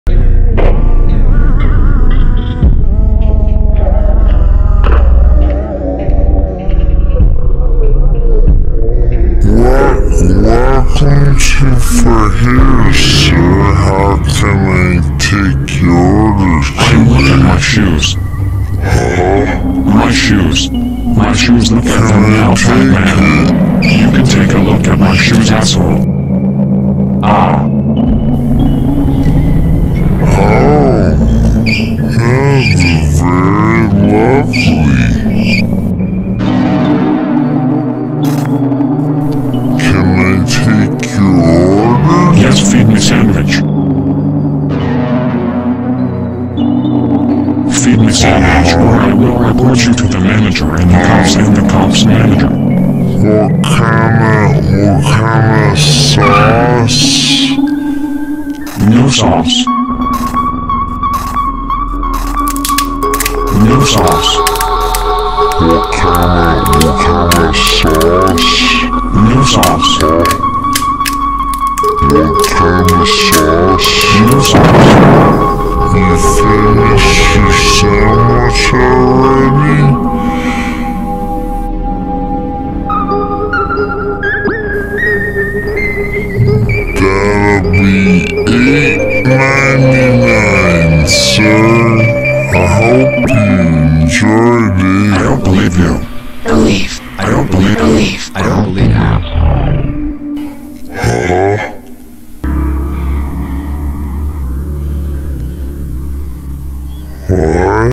Forhere's "No Sauce" - By Justin Parker (and A.I. programs) - Creepy Cartoon
Finally a new episode of Forhere's! WOW!

By Justin Parker and a couple of Artificial Intelligence Applications.

In this episode we learn what happens when you order a sandwich with no sauce. Oh boy, Oh boy!

Features music by Stir Crazy for the first 10 seconds or so, which was ruined to fit the cartoon, then the rest of the music was composed by Justin Parker at 5 in the morning while half awake and constantly harassed by his hungry house feline.

This project took many hours to create, and became something completely different than expected.